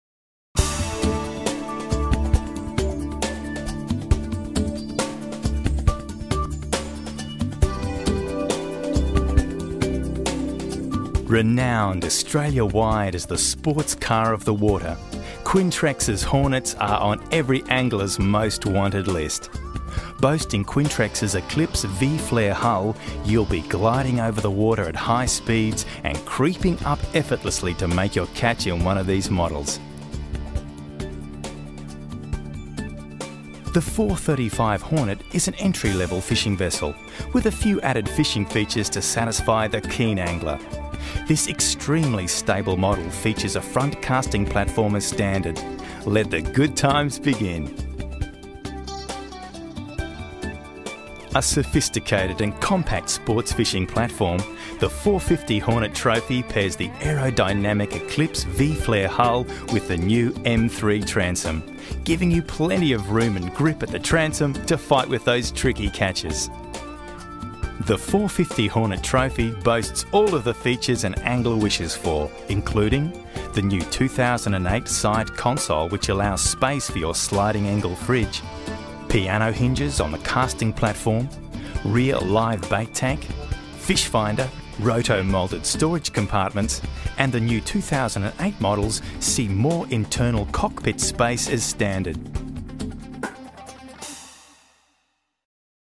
Quintrex Hornets Boat Range
Unique to Quintrex and renowned Australia-wide as the sports car of the water, Quintrex's hornets are on every angler's "most wanted" list!